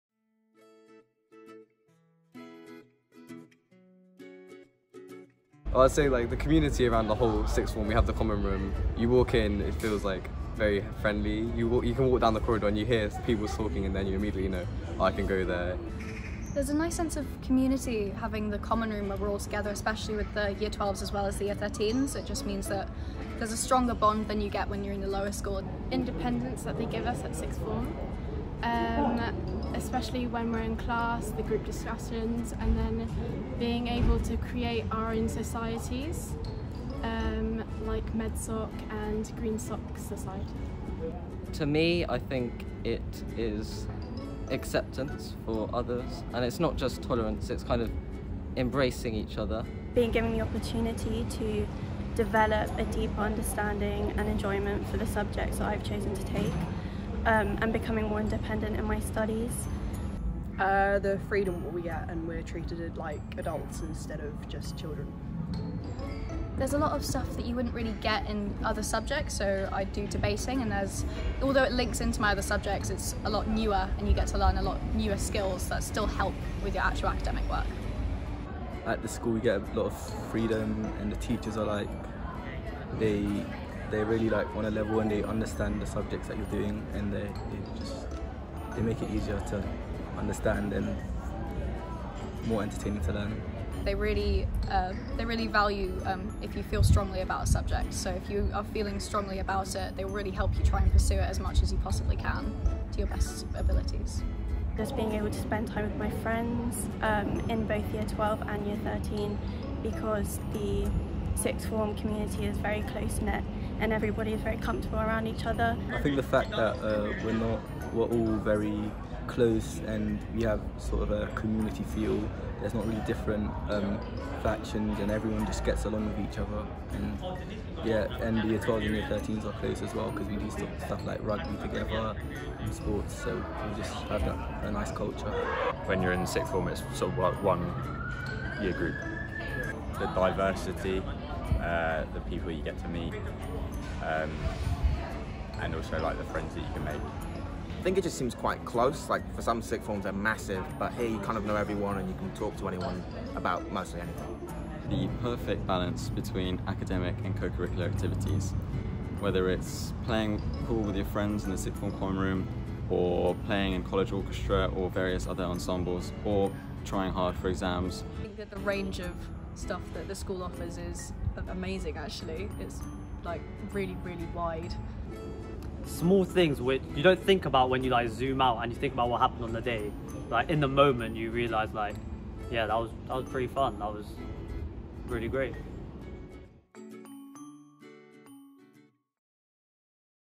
St Dunstan's College - What makes our Sixth Form special?
Our Sixth Form students speak about what makes St Dunstan's Sixth Form so special.